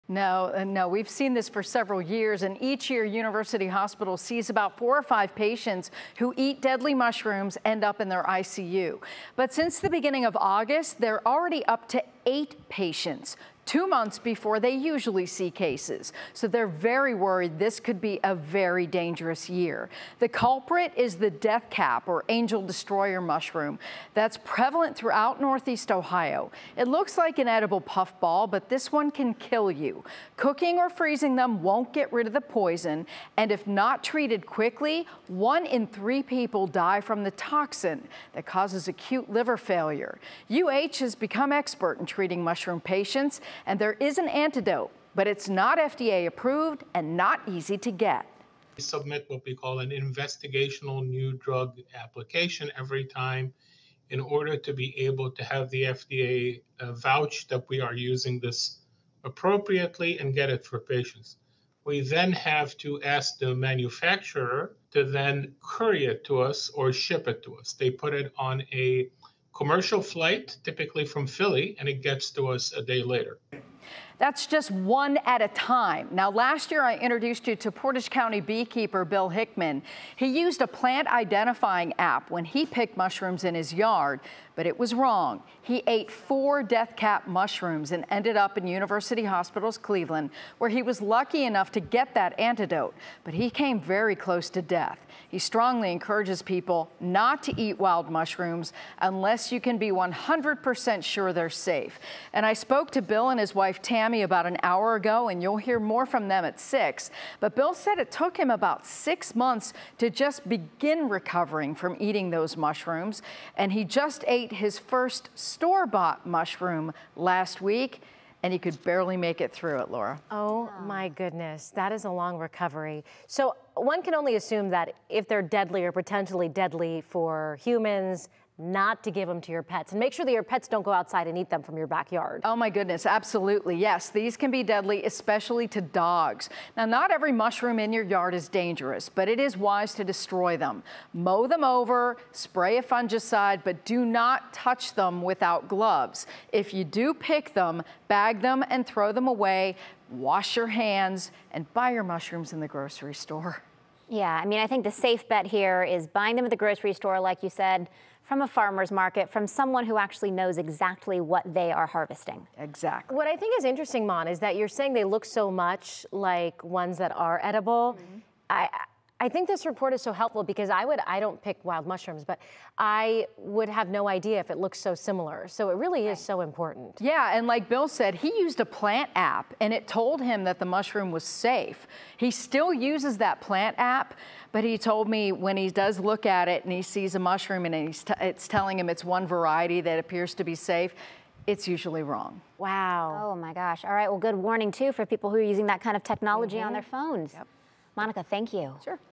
Local doctors warn against eating wild mushrooms
Local doctors are warning against eating wild mushrooms, as some are deadly. Monica Robins has the latest. --